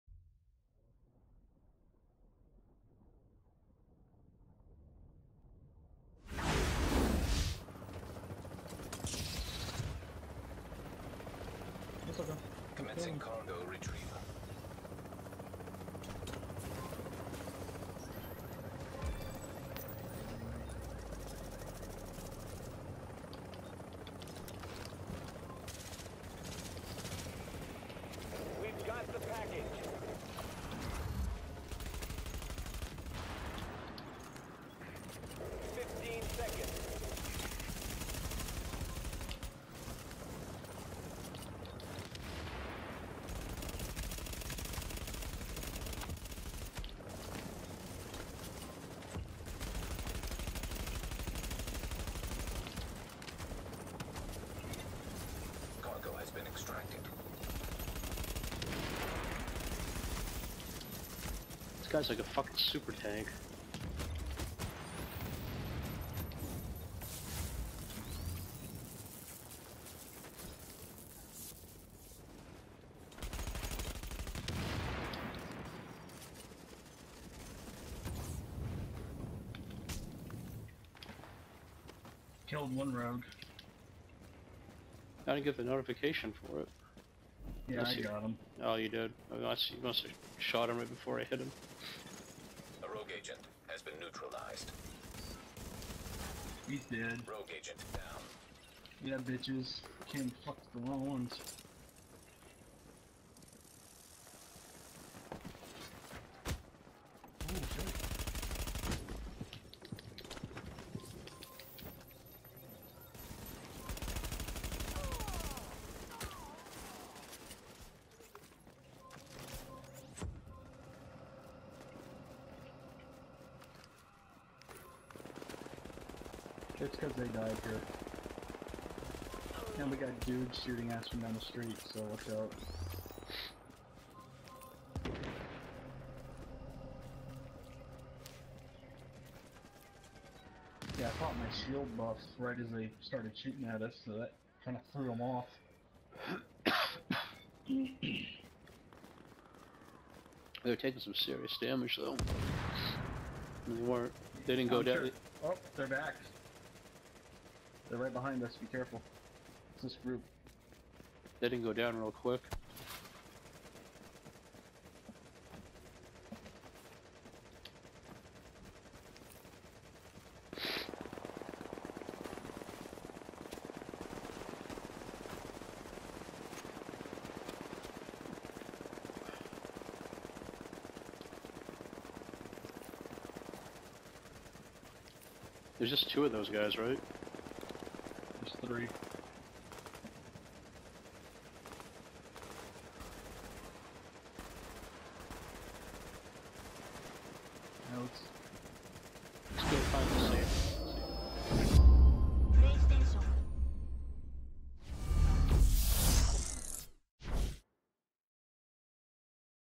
GOM Clan Dispatching of Rogues
While extracting a group of Rogues thought it was a good idea to mess with the Grump Old Men. Did not turn out very well for them.